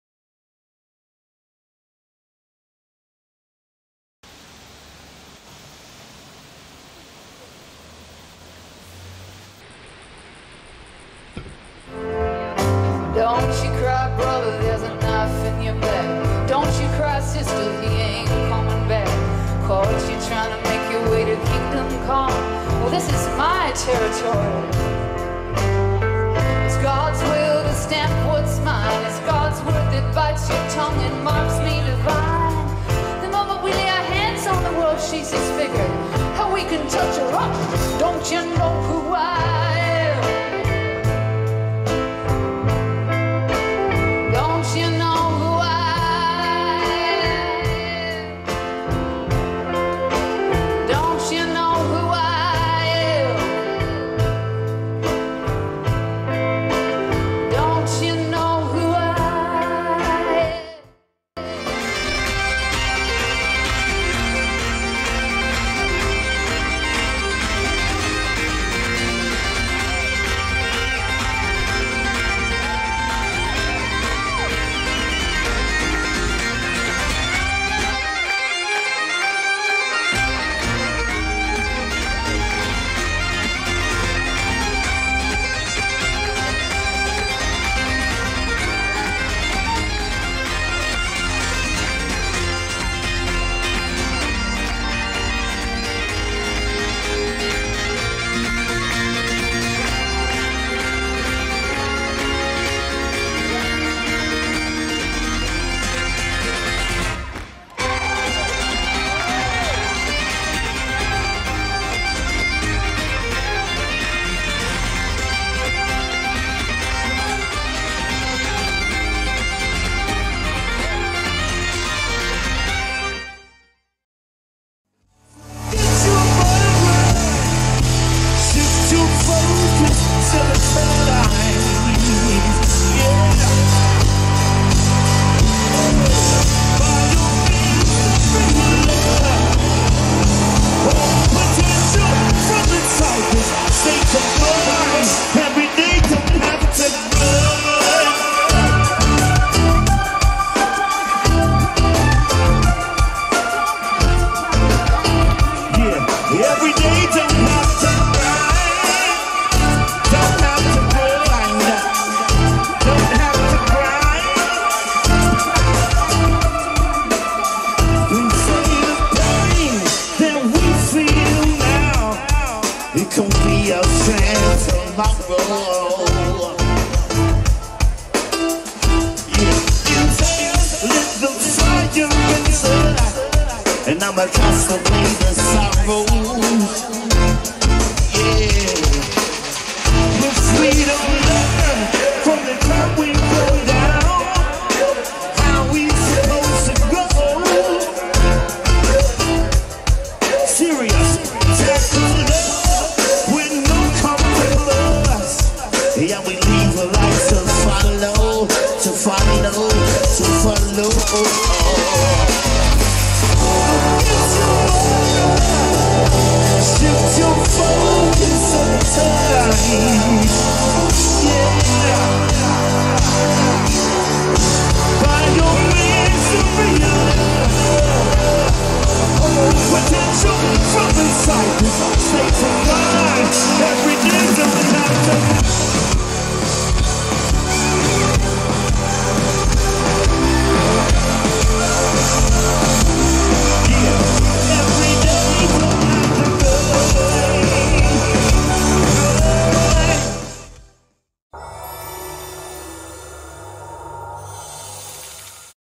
Womad 2020 Day 1
"The pain that we feel now, can be our strength tomorrow"

Reb Fountain at the Gables. Such good material, so well rehearsed. We sure know who she is
Rura lightened things up big time. We smiled as one
Shapeshifter showed us who da boss